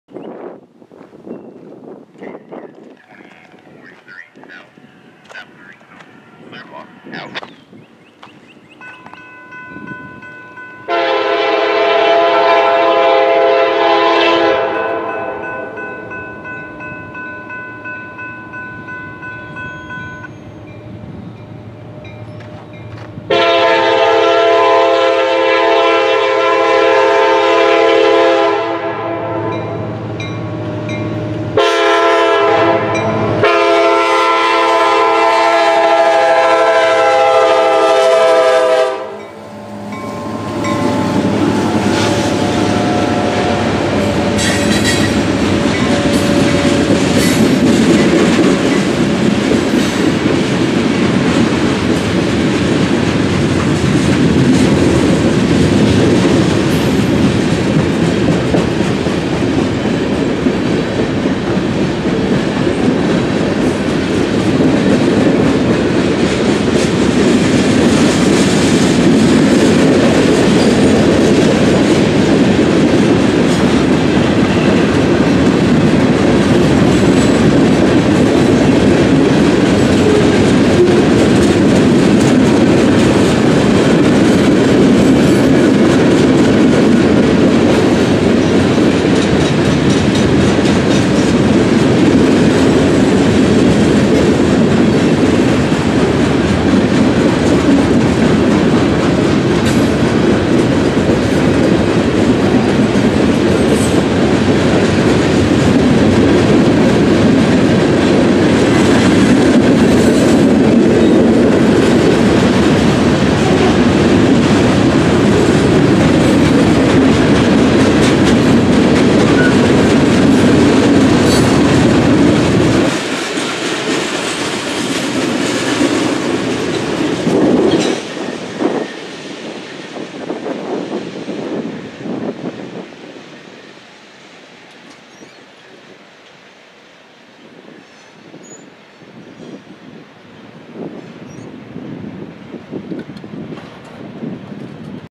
KCS YBM100 and Old Highway 62 Railroad Crossing | Mauriceville, TX | 4-24-2021
Went out to Orange County since I don't go there often and happened to catch the KCS local in Mauriceville. This crossing is at the site where a KCS train derailed last October, link to a local news article below. I believe the signal on the opposite side was taken out during the derailment and was therefore replaced. Hope you enjoy!

Crossing DOT#: 329457H

©Southeast Texas Sirens and Rail 2021

This video is protected under copyright law. Any copying of this video of any kind is unlawful without permission.